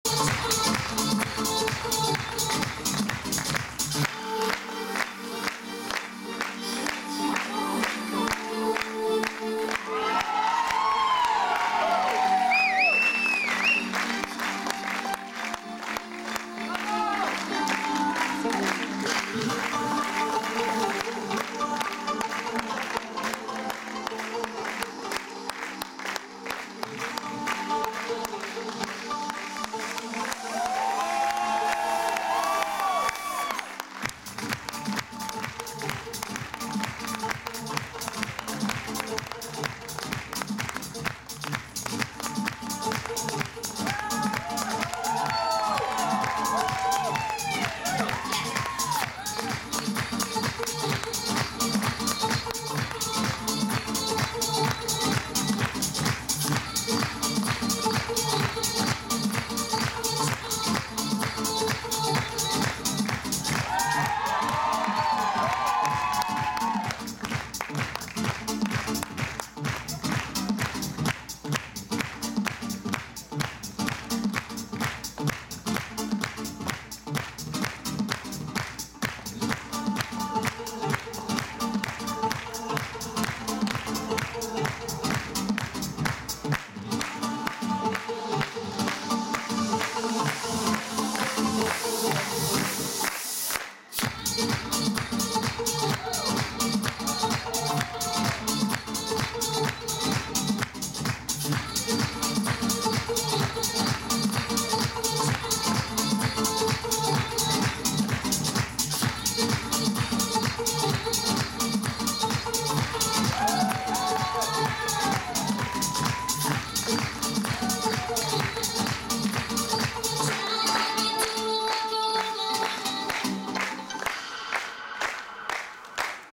olivier rosalie plan mérite avril 2011.MTS
obtention du plan merite niveau 1 , la clé qui ouvre les portes du profit share